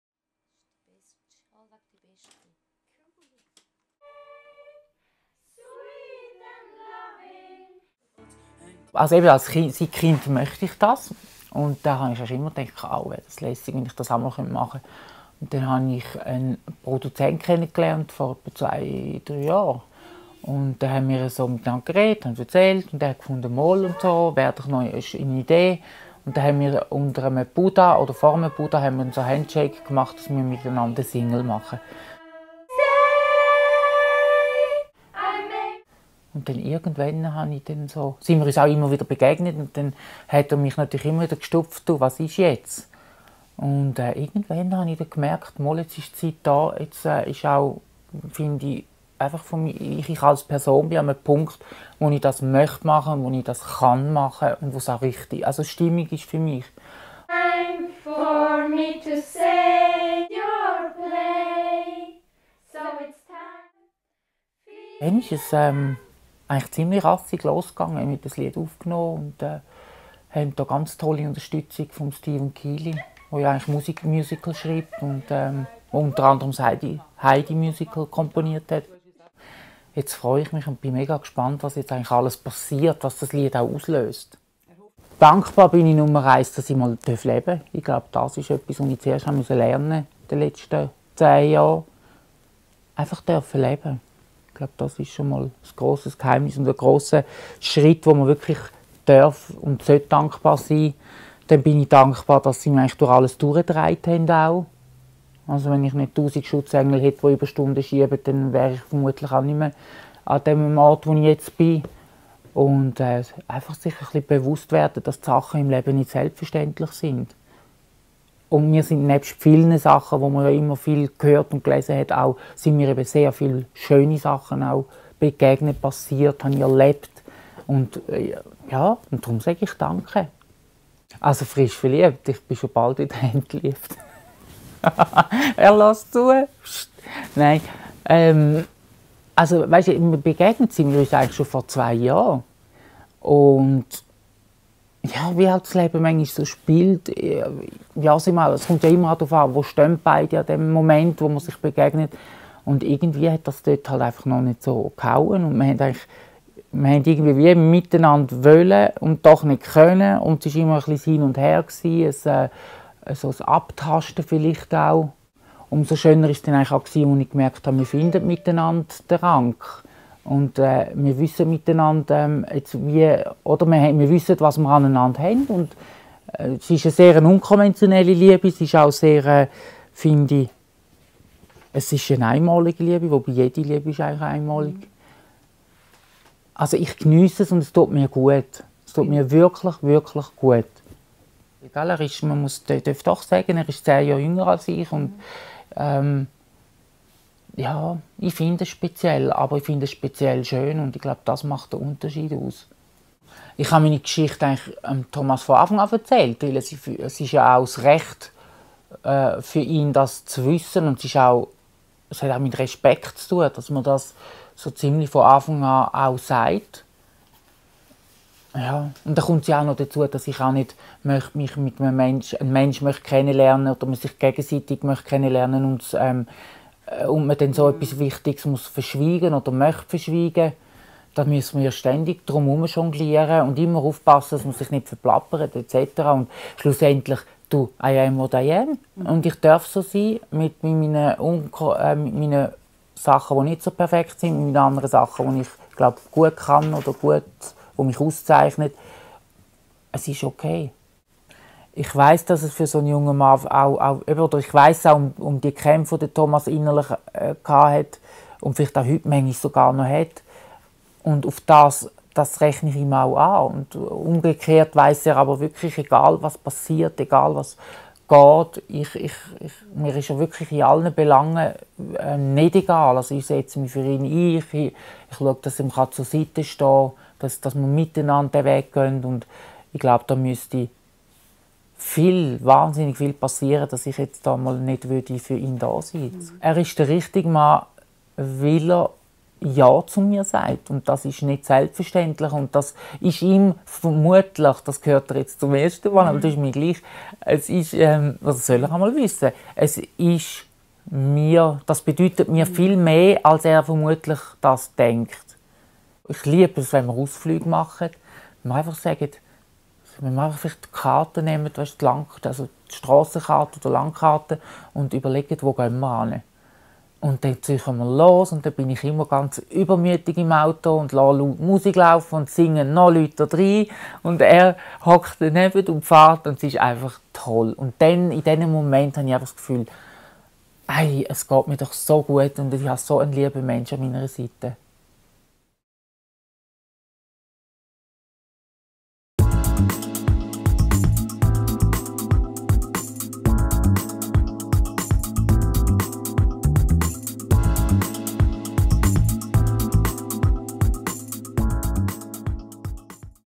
Endlich ein Mann, der zu ihr steht
- Nadia Brönimann - Die Transsexuelle Nadia Brönimann kämpfte lange für ihr Glück. In Thomas Voigt hat sie endlich einen Mann gefunden, der sie so liebt, wie sie ist. - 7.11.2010